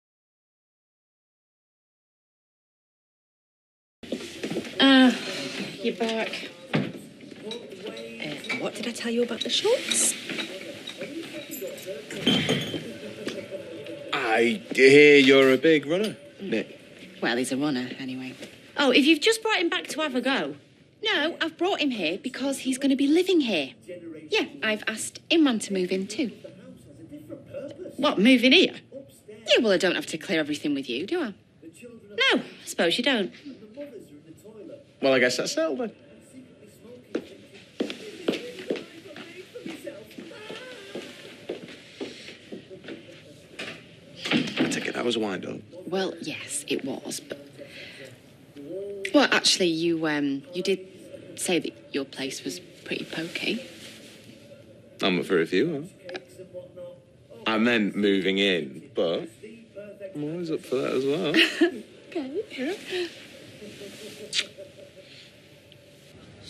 Georgia Taylor (Toyah Battersby) - Red Tights - 10/12/18
Super hot TV actress Georgia Taylor looking absolutely gorgeous in sexy red tights!

10/12/18